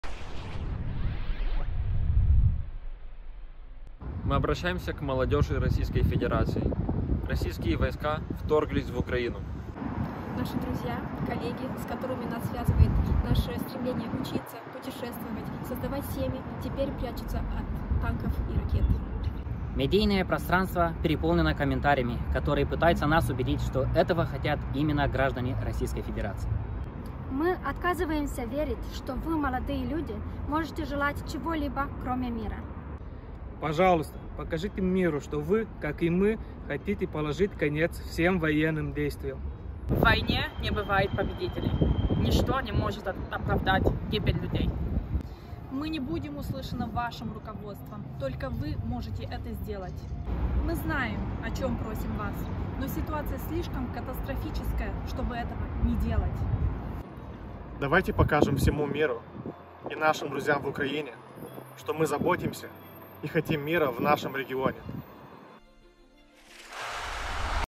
Appeal to the youth of the Russian Federation - Обращение к молодежи Российской Федерации
EDYN Moldova appeals to the youth of the Russian Federation.